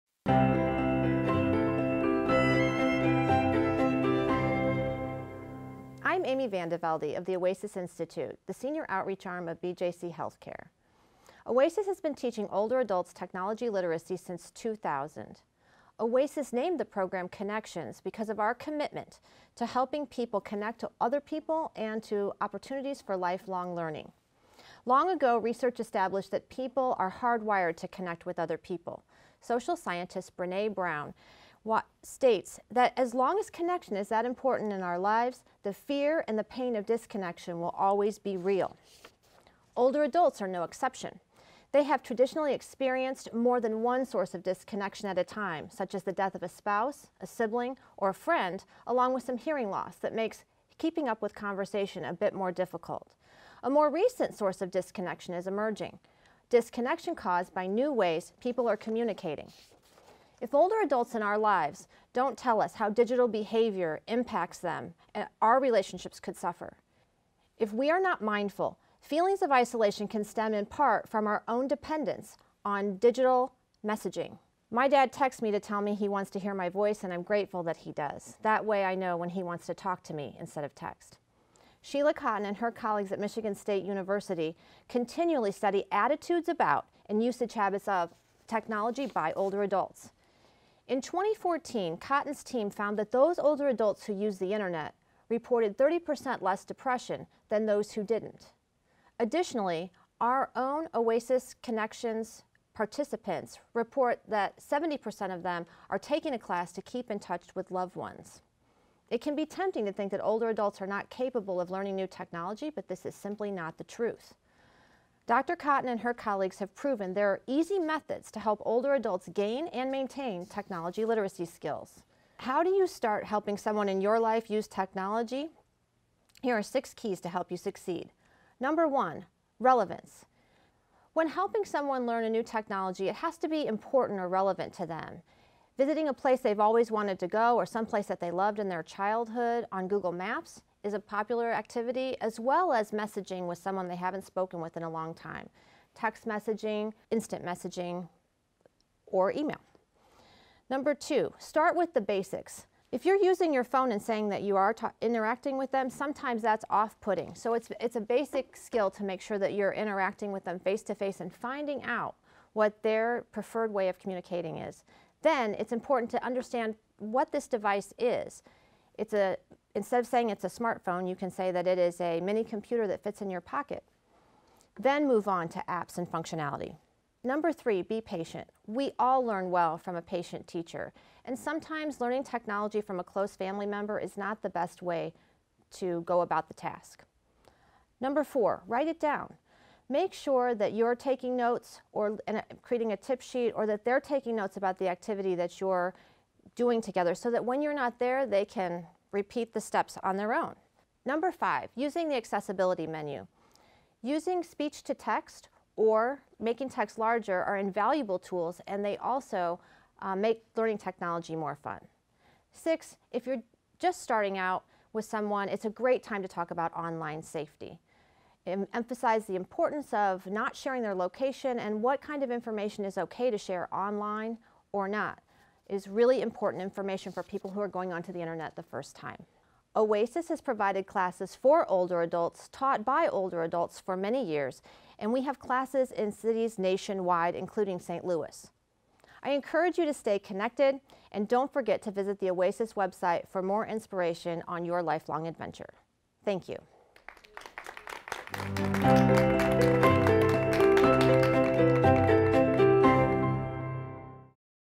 6 Tips for Helping an Older Adult Learn to Use Technology
Hear Amy VanDeVelde, national technology education manager at the Oasis Institute, talk about the positive effects technology such as the internet and smart devices has on older adults, and learn how to help them become connected.